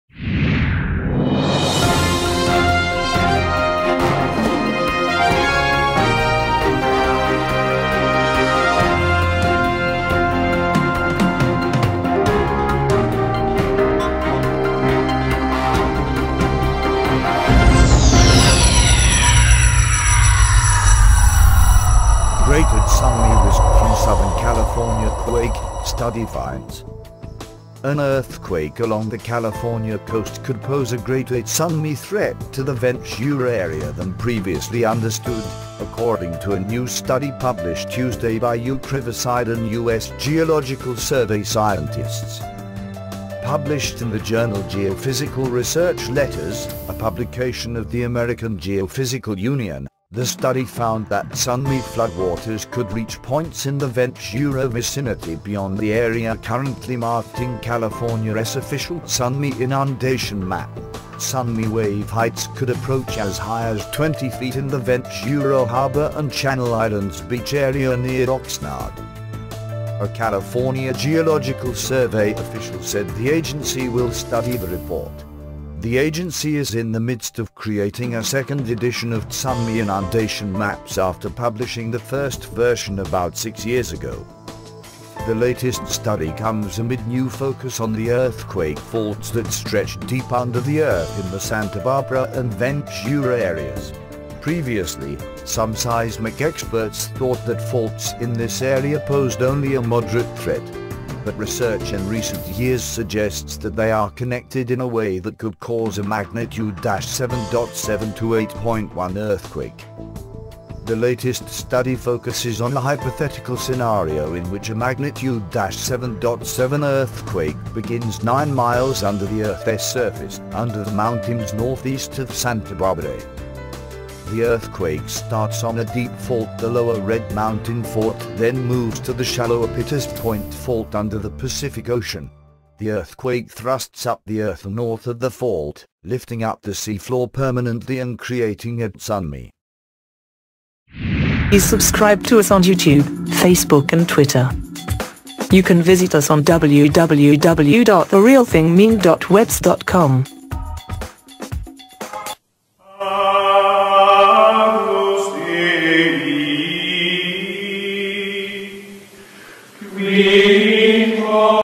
Greater tsunami risk from Southern California quake, study finds
An earthquake along the California coast could pose a greater tsunami threat to the Ventura area than previously understood, according to a new study published Tuesday by UC Riverside and U.S. Geological Survey scientists.

Published in the journal Geophysical Research Letters, a publication of the American Geophysical Union, the study found that tsunami floodwaters could reach points in the Ventura vicinity beyond the area currently marked in California’s official tsunami inundation map. Tsunami wave heights could approach as high as 20 feet in the Ventura Harbor and Channel Islands Beach area near Oxnard.

A California Geological Survey official said the agency will study the report. The agency is in the midst of creating a second edition of tsunami inundation maps after publishing the first version about six years ago.

The latest study comes amid new focus on the earthquake faults that stretch deep under the earth in the Santa Barbara and Ventura areas. Previously, some seismic experts thought that faults in this area posed only a moderate threat. But research in recent years suggests that they are connected in a way that could cause a magnitude-7.7 to 8.1 earthquake.

The latest study focuses on a hypothetical scenario in which a magnitude-7.7 earthquake begins nine miles under the Earth’s surface, under the mountains northeast of Santa Barbara.

The earthquake starts on a deep fault – the Lower Red Mountain fault – then moves to the shallower Pitas Point fault under the Pacific Ocean. The earthquake thrusts up the earth north of the fault, lifting up the seafloor permanently and creating a tsunami.